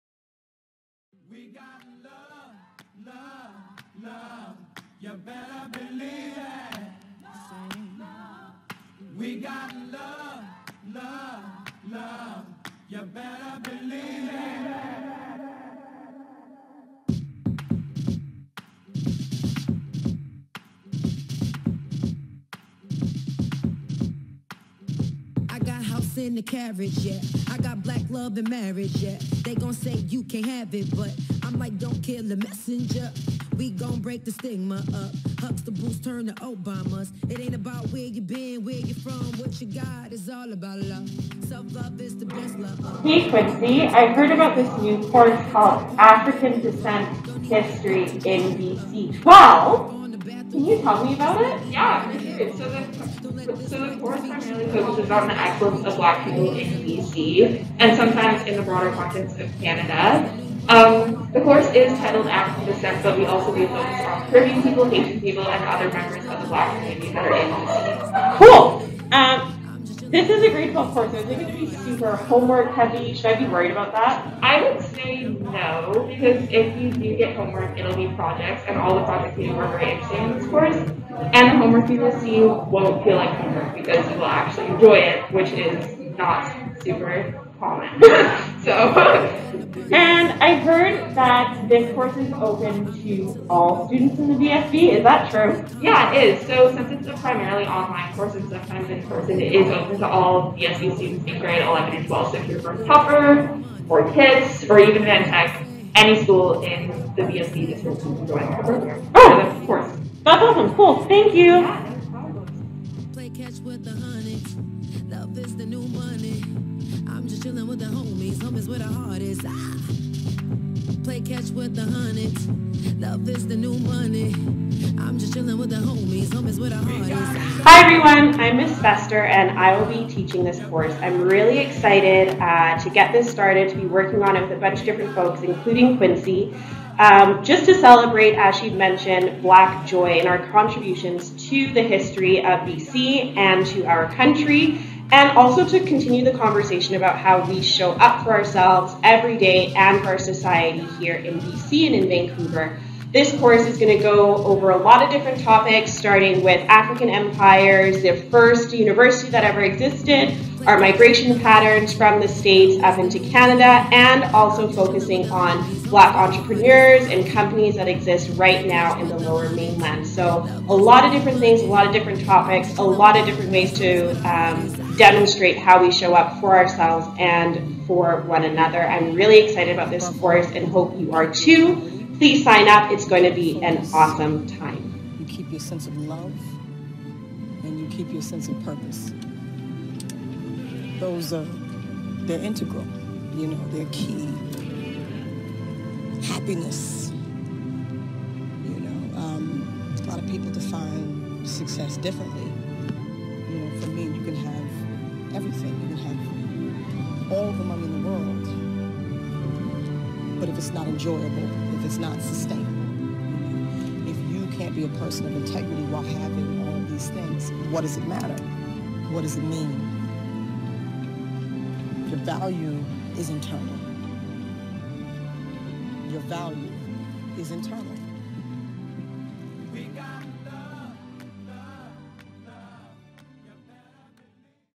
African Descent History in BC 12
African-Canadian history is Canadian history. This course will highlight the missing pages of Black history in BC and will provide our diverse community with the tools to examine it within the context of African Descent Canadian
History. It will include an overview of the Transatlantic slave trade when Africans were first brought to Canada in the 17th and 18th centuries. However, the African Descent History course will mainly focus on the Black pioneers in BC, beginning with Sir James Douglas and migration of hundreds of African American families to BC in 1858. This course will also explore their eventual journeys to other parts of Canada, such as the prairies and Newfoundland, focusing on significant historical experiences and the valuable contributions of Black Canadians. Emphasis will also
be placed on the complexity and richness of the contemporary Black community in BC.